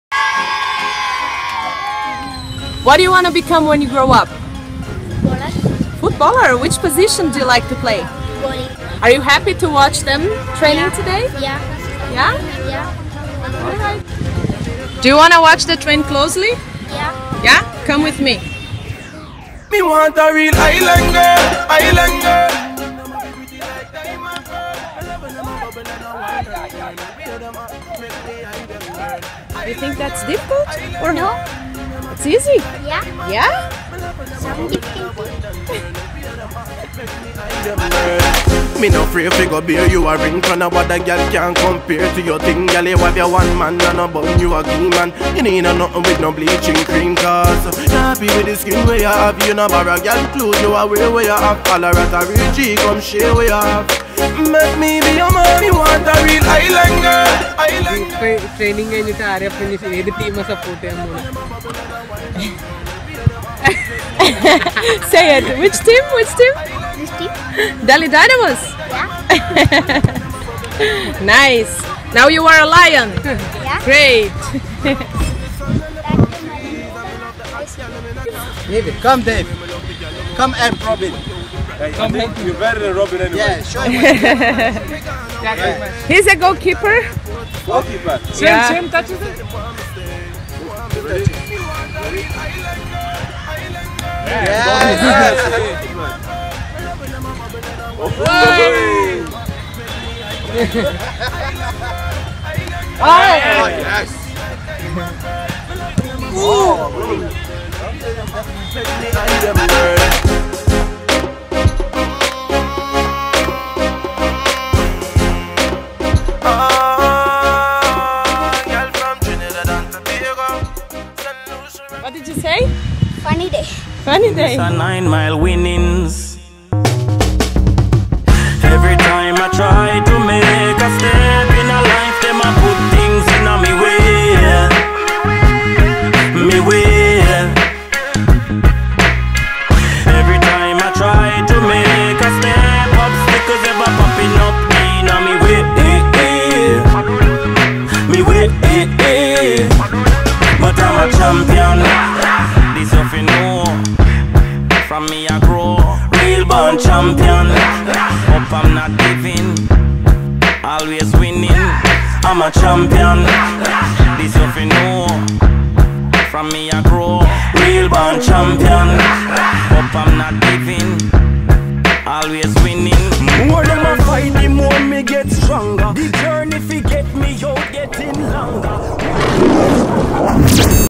John Arne Riise & Florent Malouda surprise a young Kerala Blasters fan
Delhi Dynamos FC are known for playing roaring football on the pitch, and giving back to the community. It's no different when the Lions travel away from The Den. Our Lions Robin Singh, Florent Malouda and John Arne Riise made the dream of a young footballer come true in Kochi. ‪#‎RoarWithTheLions‬.